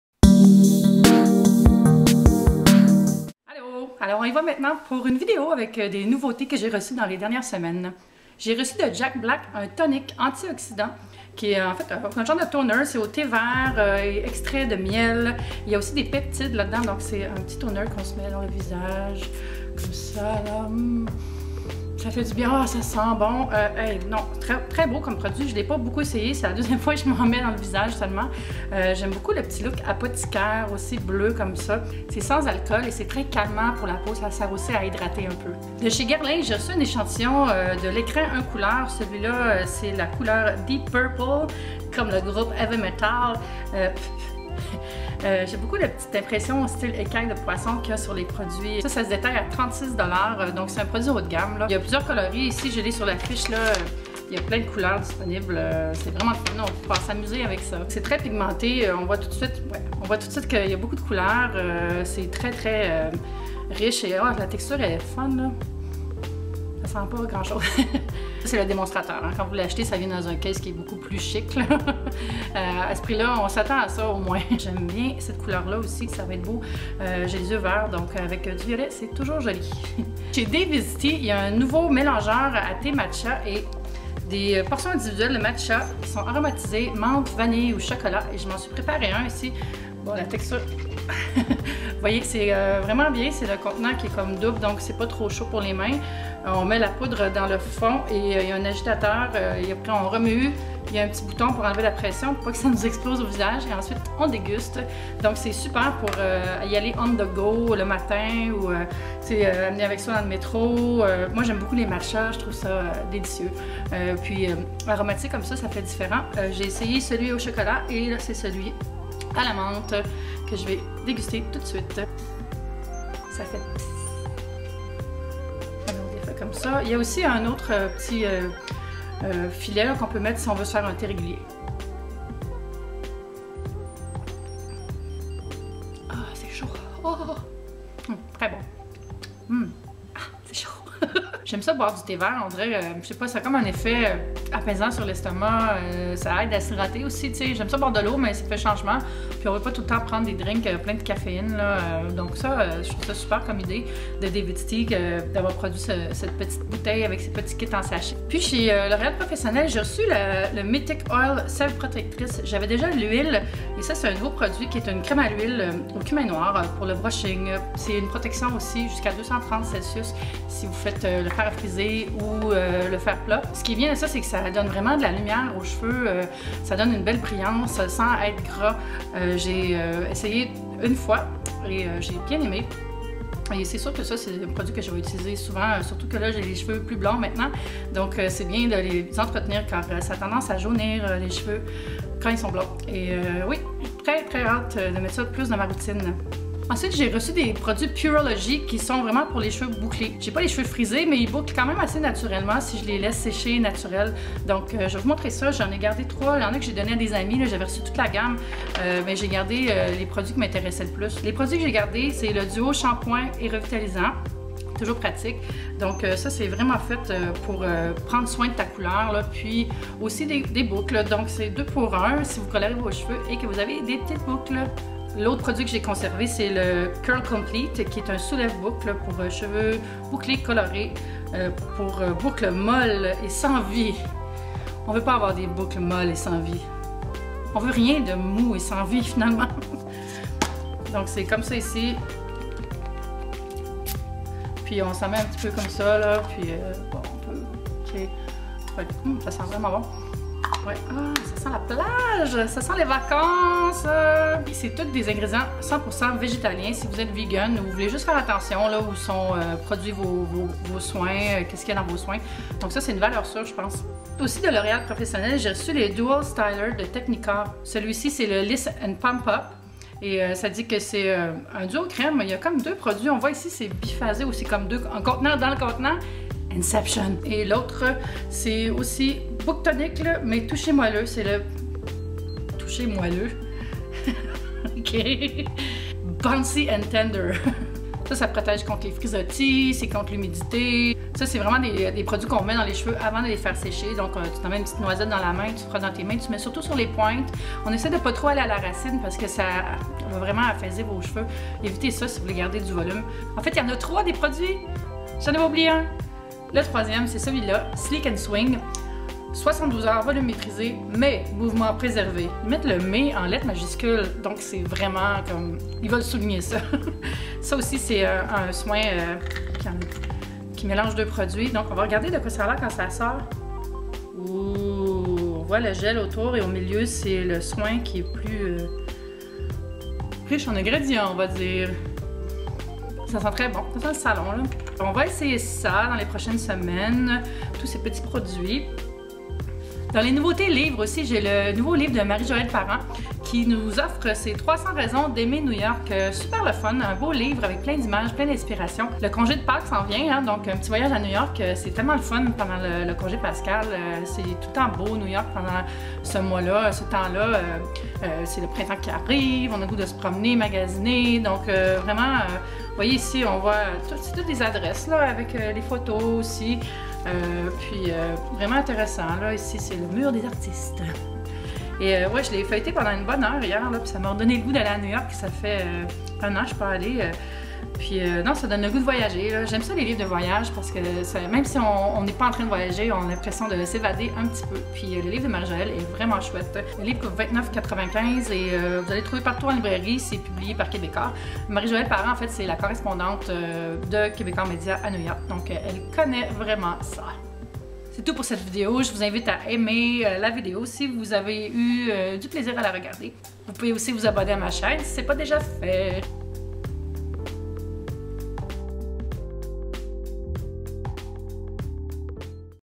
WOW! || Les nouveautés de mars!
YEAH! J'ai plein de produits à partager avec vous. →→ THUMBS UP pour cette vidéo si elle vous a plu. Merci!

Dispo chez Sephora, La Baie et Holt Renfrew
24$

36$

Mélangeur à thé matcha DAVIDsTEA (34,50$)

26$

Prix variés

(27$)

300 raisons d’aimer New York par Marie-Joëlle Parent (29,95$)

POUR ME PARLER
Je réponds à vos questions dans la section commentaires des vidéos ☺